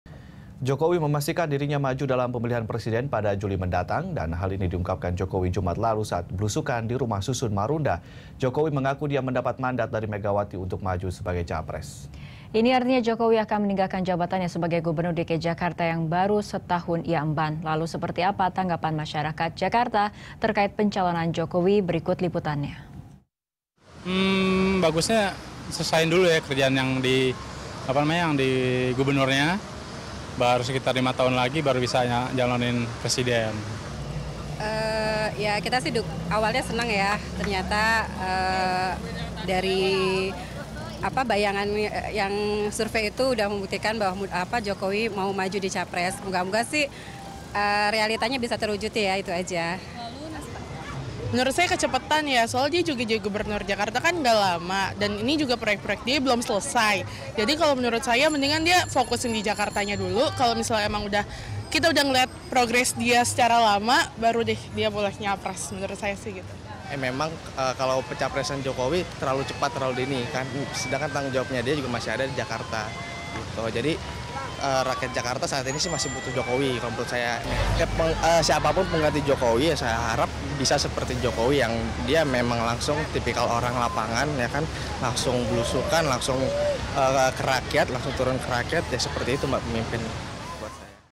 Reaksi Publik Terhadap Pencapresan Jokowi
Gubernur DKI Jakarta Jokowi memastikan dirinya maju dalam pemilihan presiden pada Juli mendatang. Hal ini diungkapkan pemilik nama lengkap Joko Widodo itu, Jumat lalu, saat blusukan di Rumah Susun Marunda, Jakarta Utara.

Facebook.com/BeritaSatuTV
Youtube.com/BeritaSatu